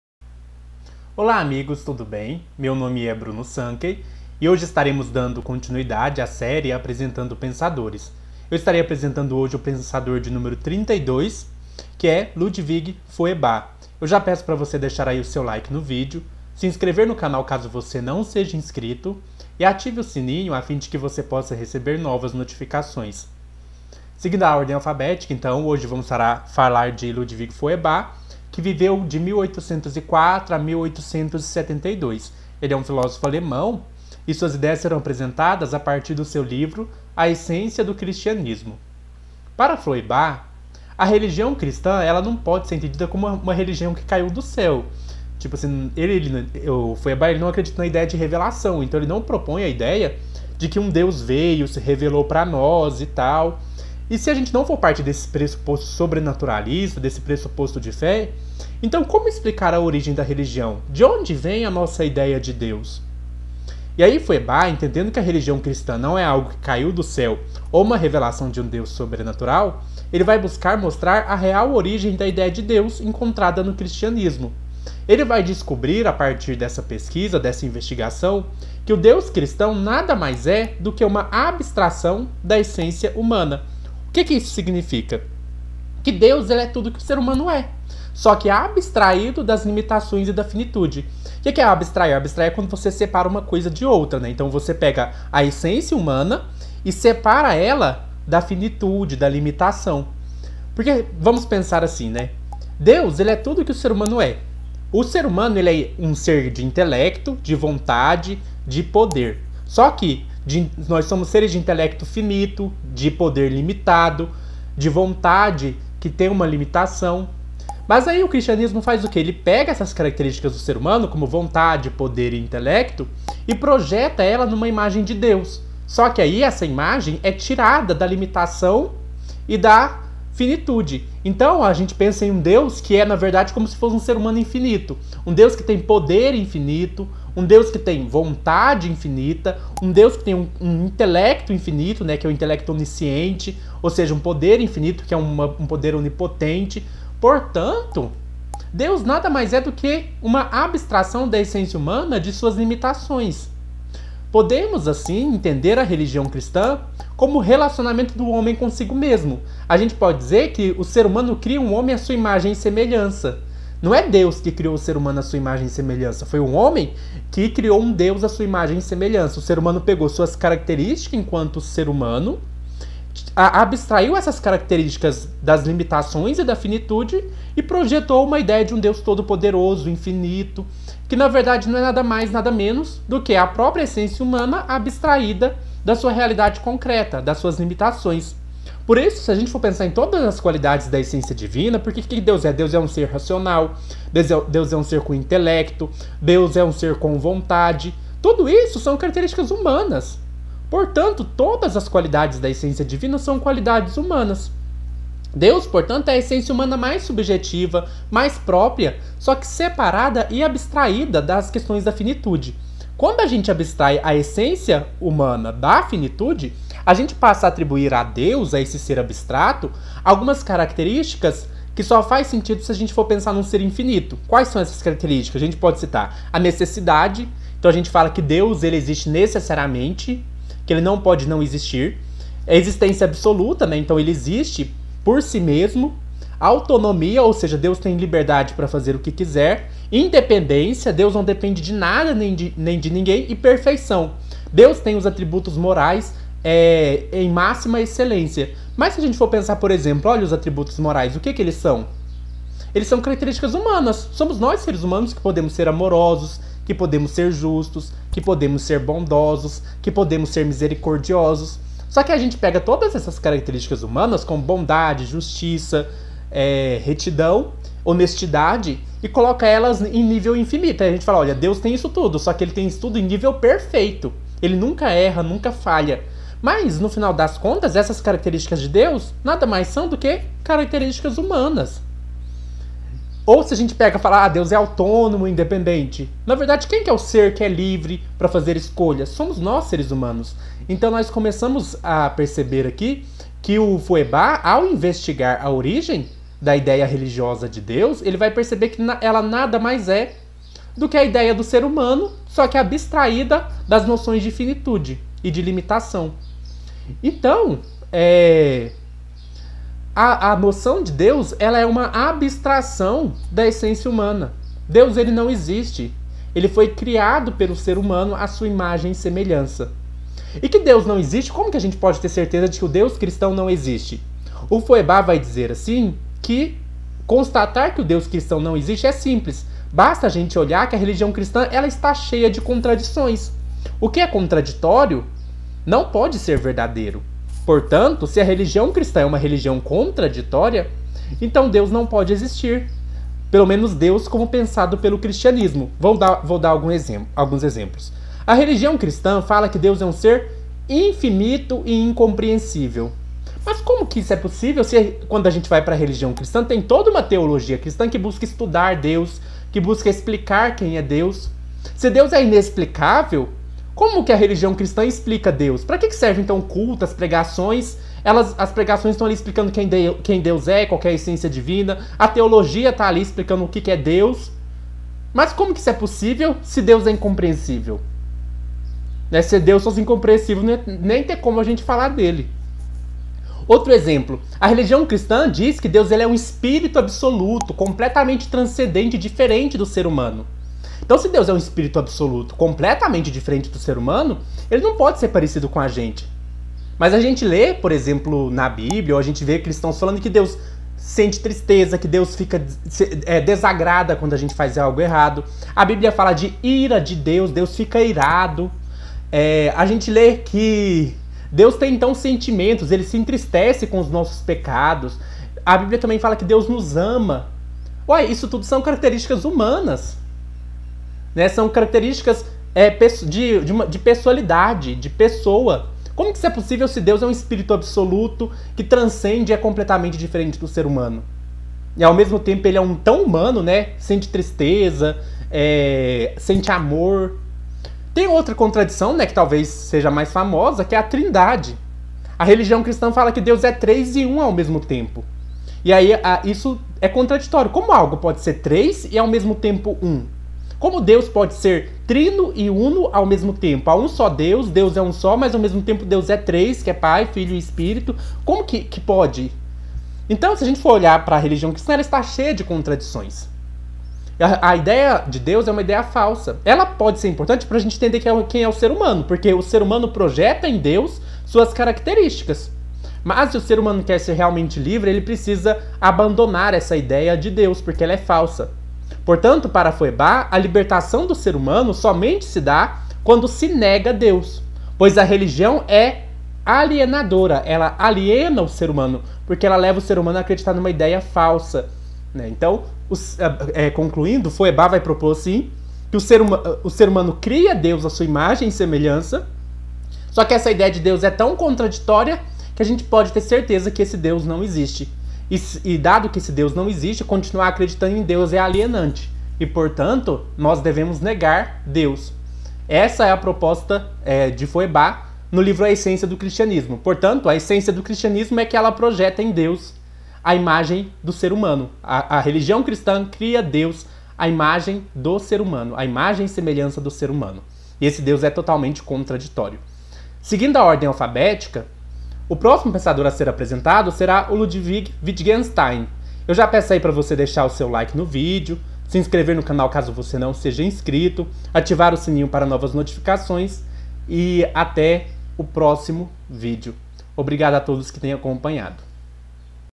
PENSADOR 32: LUDWIG FEUERBACH - A ESSÊNCIA DO CRISTIANISMO
Hoje é a vez do filósofo alemão Ludwig Feuerbach (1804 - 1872), cujas ideias serão apresentadas a partir do seu livro A Essência do Cristianismo. Entendendo que a religião cristã não é algo que caiu do céu ou uma revelação de um Deus sobrenatural, Feuerbach busca mostrar a real origem da ideia de Deus encontrada no Cristianismo. Ele descobre, então, que o Deus cristão nada mais é do que uma abstração da essência humana. Deus é tudo que o ser humano é, só que abstraído das limitações e da finitude. O humano é um ser intelectual, com vontade, poder e intelecto, mas finito e limitado. O Cristianismo, então, abstrai o humano de sua realidade concreta e projeta a ideia de um ser com intelecto, vontade e poder, mas não sujeito à finitude ou limitação. Podemos, assim, entender a religião cristã como o relacionamento do homem consigo mesmo, com sua própria essência, mas como se fosse uma outra essência. A essência divina nada mais é do que a essência humana, abstraídas as limitações do humano concreto, real, corporal e objetivado. Por isso, todas as qualidades da essência divina são qualidades humanas. Deus é a essência humana mais subjetiva, mais própria, separada e abstraída. A esse "Deus", criado por meio dessa abstração, é atribuído necessidade, existência absoluta, autonomia, independência e perfeição. Mas que o Deus cristão não existe, é evidente pelo fato de que a religião cristã é cheia de contradições. Por exemplo, Deus é visto como um ser infinito e incompreensível, mas ao mesmo tempo há toda uma teologia cristã explicando como Deus é. Outra contradição: a religião cristã diz que Deus é um Espírito Absoluto completamente transcendente e diferente do ser humano e ao mesmo tempo diz que esse Deus é um ser individual e pessoal, que se entristece com nossos pecados, que ama, etc. Mais uma crença cristã contraditória é a doutrina da Trindade, que diz que Deus é um ser uno e um só e ao mesmo tempo que ele é três pessoas. Feuerbach conclui dizendo que a religião é um sonho da mente humana, de modo que as imagens religiosas são como os sonhos, símbolos importantes para compreensão da essência humana. Para ele, a libertação do ser humano somente se dá com a negação de Deus, pois a religião é expressão e causa da alienação humana.